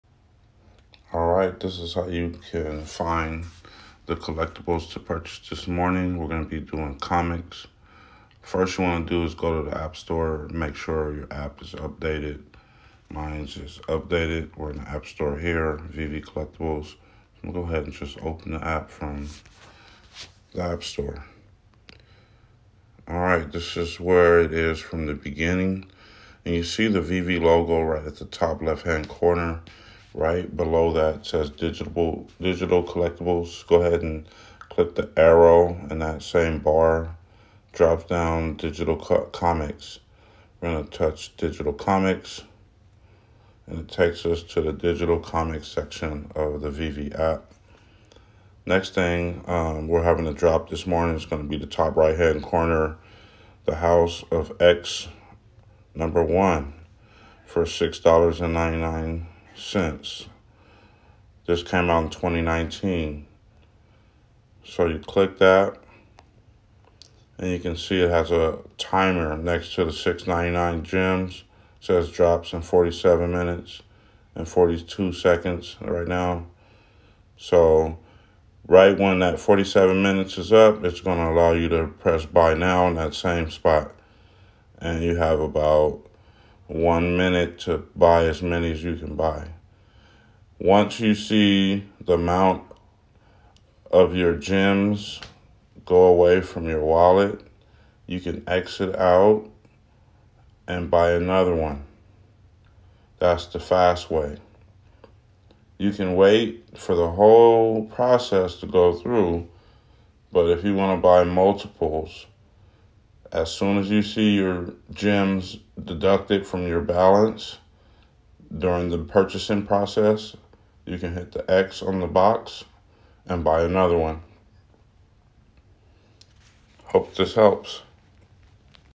Veve blind box style drop marvel digital comics
Here is how you can access the comics on the VeVe app for a blind box drop